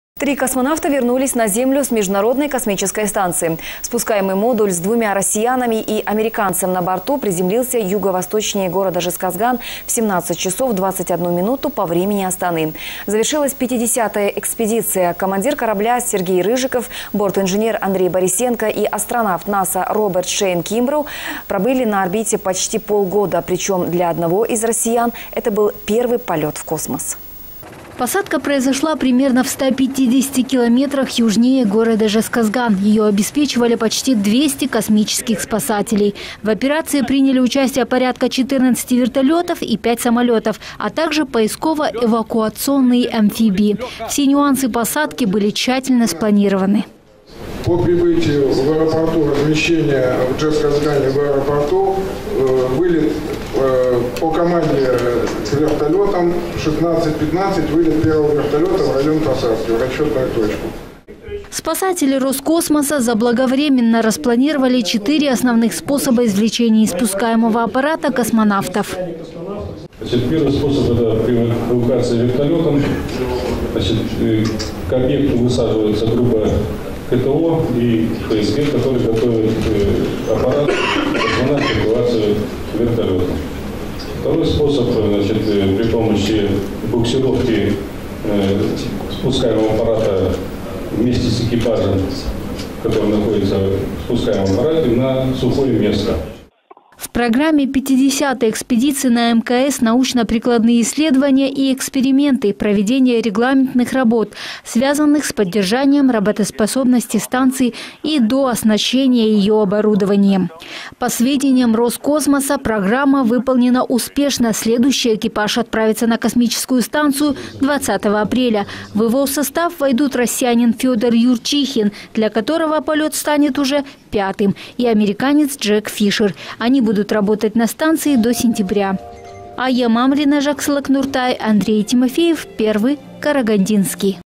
Три космонавта вернулись на Землю с Международной космической станции.
Спускаемый модуль с двумя россиянами и американцем на борту приземлился юго-восточнее города Жезказган в 17 часов 21 минуту по времени Астаны. Завершилась 50-я экспедиция: командир корабля Сергей Рыжиков, бортинженер Андрей Борисенко и астронавт NASA Роберт Шейн Кимброу пробыли на орбите почти полгода, причем для одного из россиян это был первый полет в космос.
Посадка произошла примерно в 150 километрах южнее города Жезказган.  Ее обеспечивали почти 200 космических спасателей. В операции приняли участие порядка 14 вертолетов, и пять самолетов, а также поисково-эвакуационные амфибии. Все нюансы посадки были тщательно спланированы.
Спасатели «Роскосмоса» заблаговременно распланировали четыре основных способа извлечения из спускаемого аппарата космонавтов.
В программе 50-ой экспедиции на МКС – научно-прикладные исследования и эксперименты, проведение регламентных работ, связанных с поддержанием работоспособности станции и дооснащение её оборудованием. По сведениям «Роскосмоса», программа выполнена успешно. Следующий экипаж отправится на космическую станцию 20-го апреля. В его состав войдут россиянин Федор Юрчихин, для которого полет станет уже пятым. И американец Джек Фишер. Они будут работать на станции до сентября.